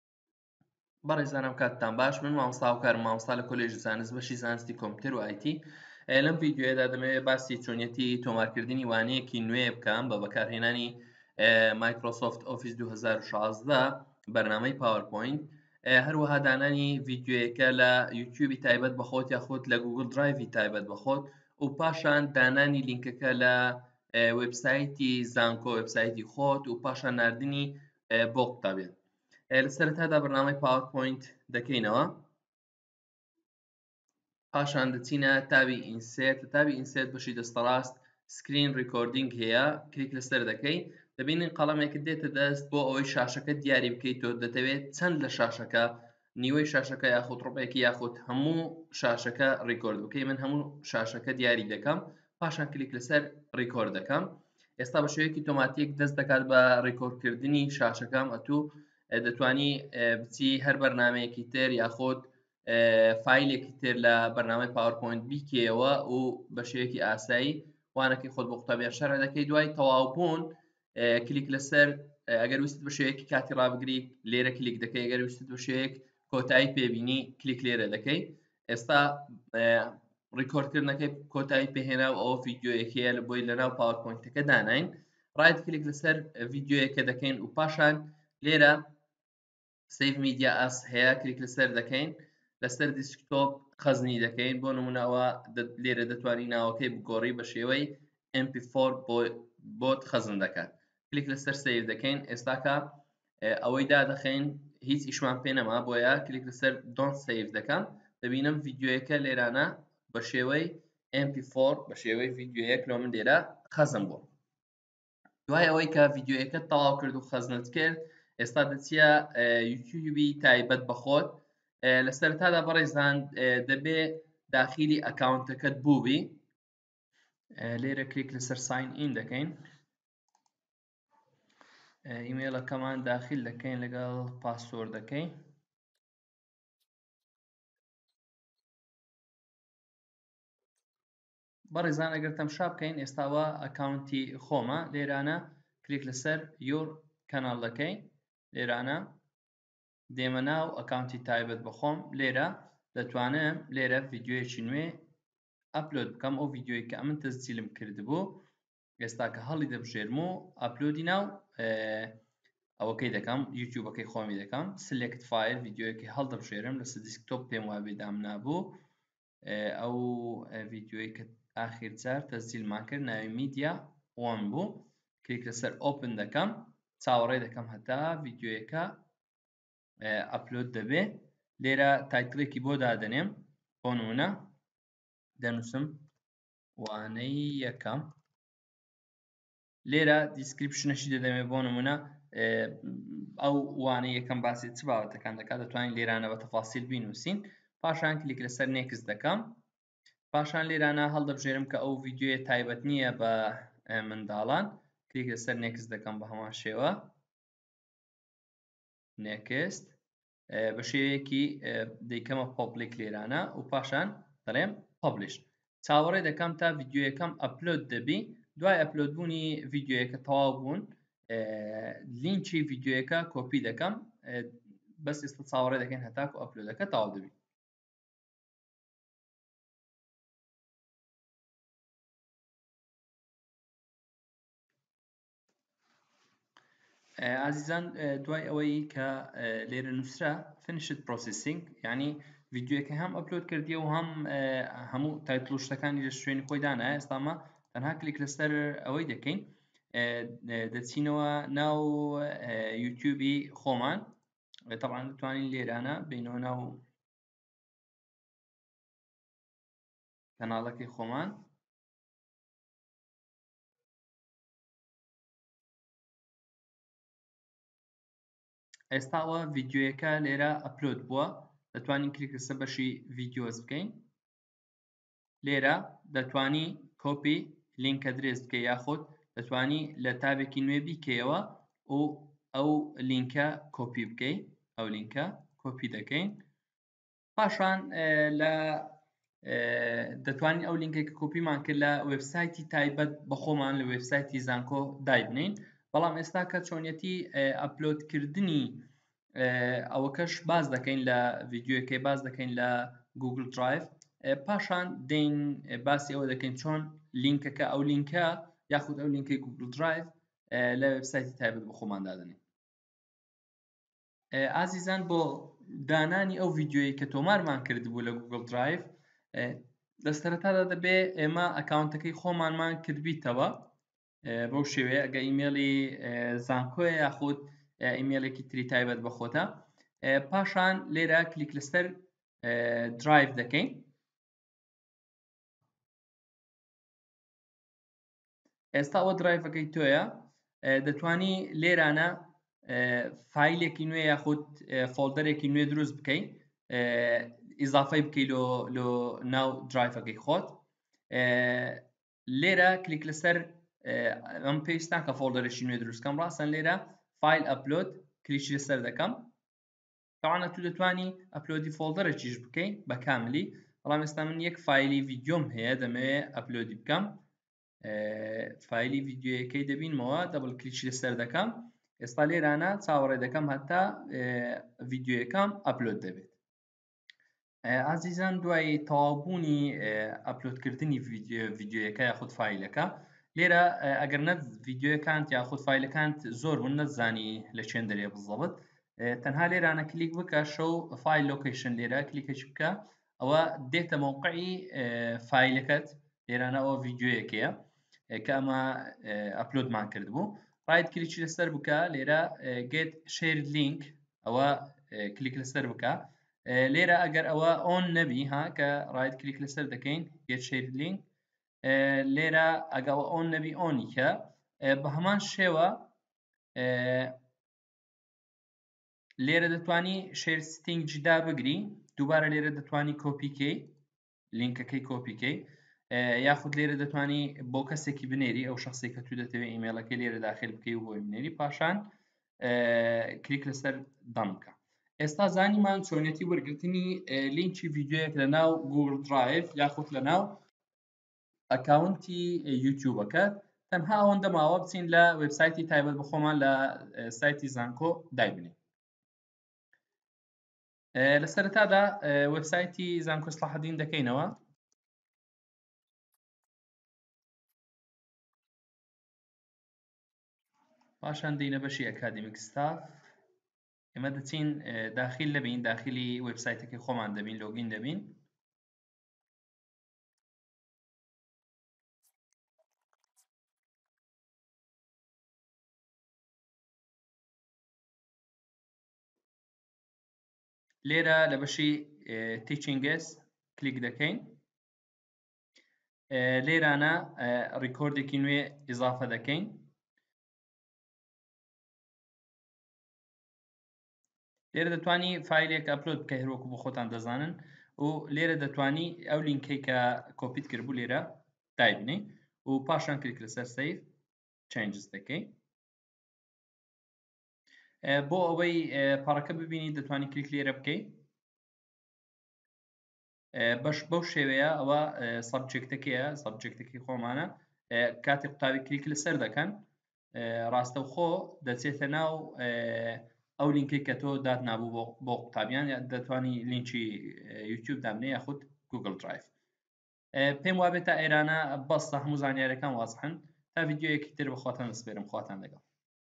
Recording New Lecture Using Powerpoint.
لەم ڤیدیۆیەدا باسی چۆنیەتی تۆمارکردنی وانەیەک دەکەین بە بەکارهێنانی پاوەرپۆین و دانانی لە یوتیوب یاخود لە گووگل درایڤ، وە دواتر دانانی لینکی ڤیدیۆیەکە لە وێبستایتی خۆت.